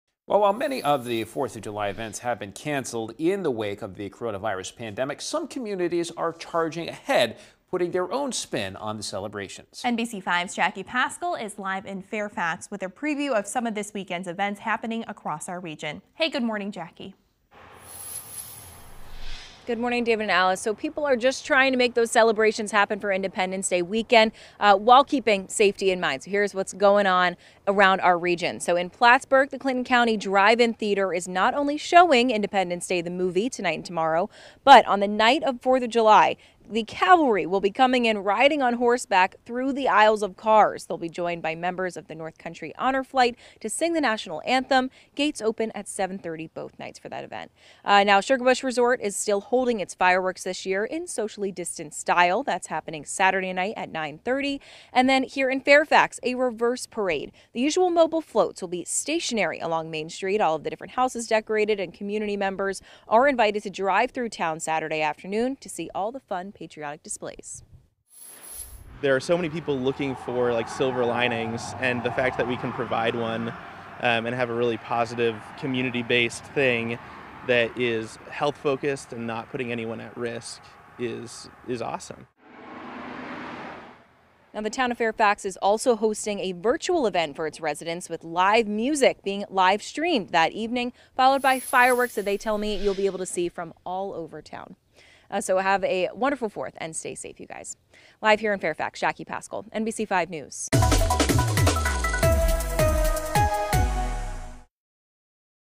Events happening in our region to celebrate Independence Day
Towns take creative approaches to July 4 celebrations amid COVID-19 pandemic